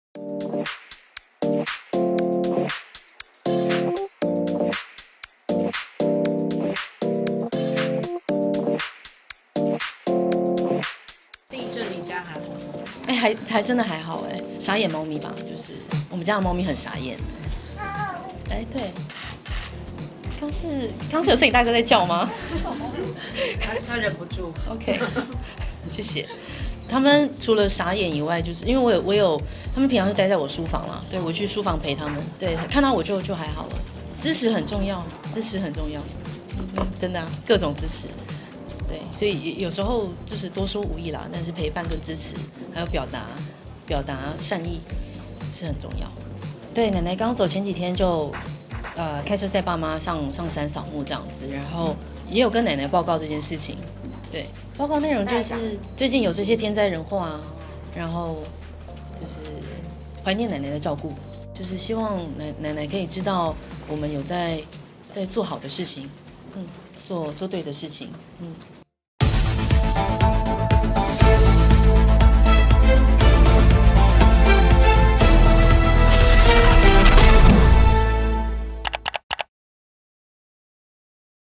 地震時陪伴家裡的「傻眼貓咪」 林予晞懷念奶奶：我們有在做對的事
林予晞昨（8日）出席品牌代言記者會，談到上週發生的地震，她表示家裡的貓咪變成「傻眼貓咪」，不過有她陪伴在旁邊就沒事了，也呼應先前的事件表示：「支持很重要，多說無益，陪伴支持、表達善意是最重要的事！」

另外她的奶奶前陣子剛過世，她和爸媽上山掃墓，林予晞有向奶奶報告近日發生的天災人禍，以及懷念奶奶的照顧，她也希望奶奶能知道她在做對的事情。